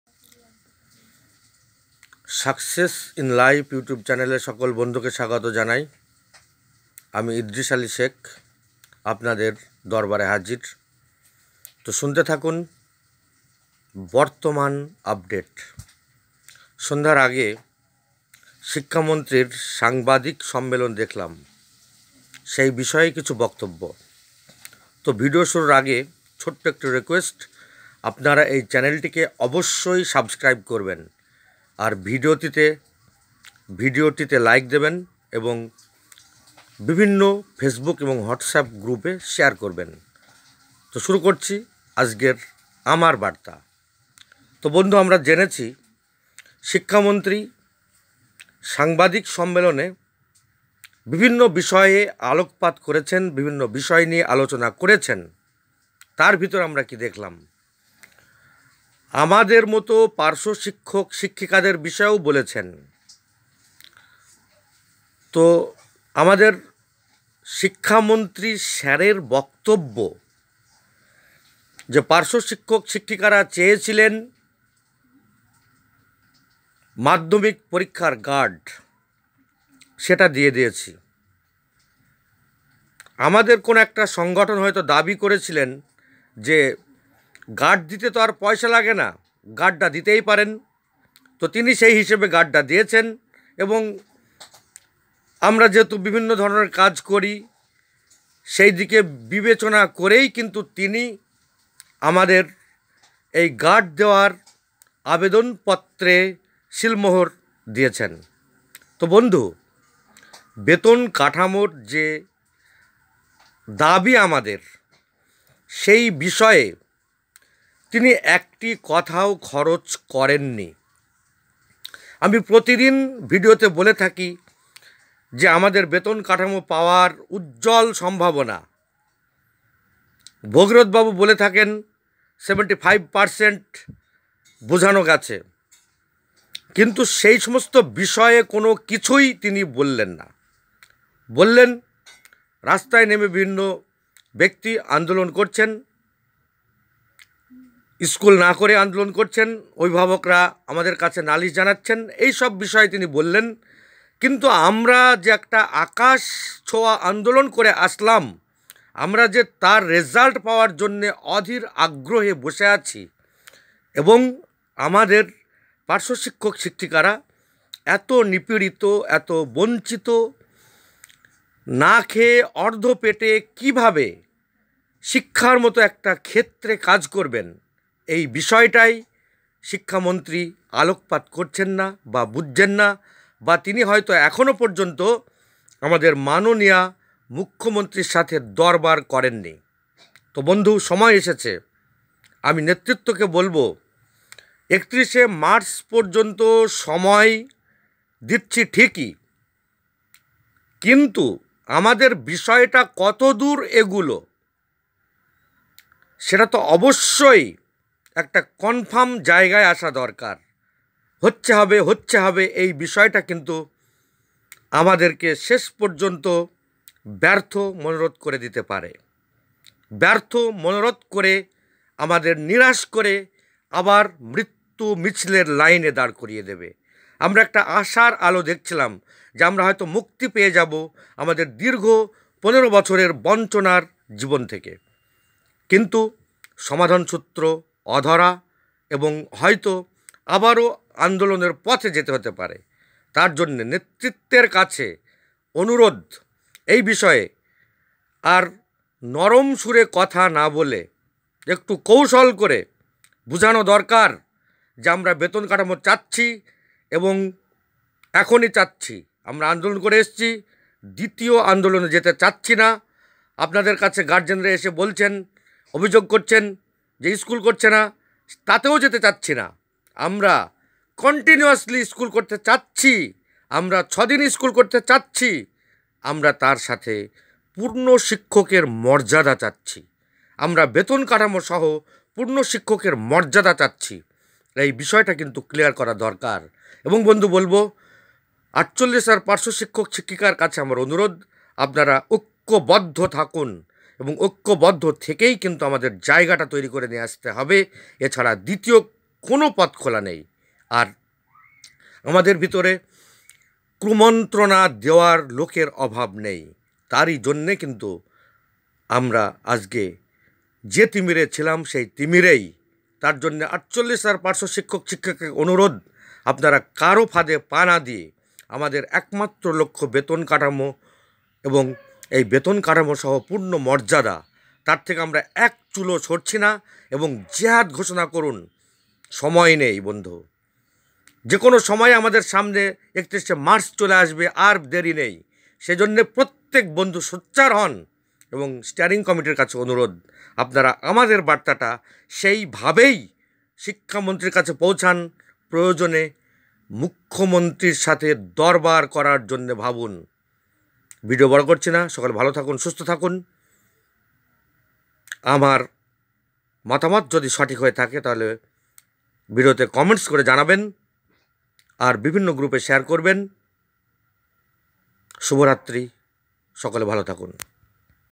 PARATEACHER BARTA* আমার বার্তা*শিক্ষামন্ত্রীর সাংবাদিক সম্মেলনে বেতন কাঠামোর কোনো খবর নেই।ভাবতে হবে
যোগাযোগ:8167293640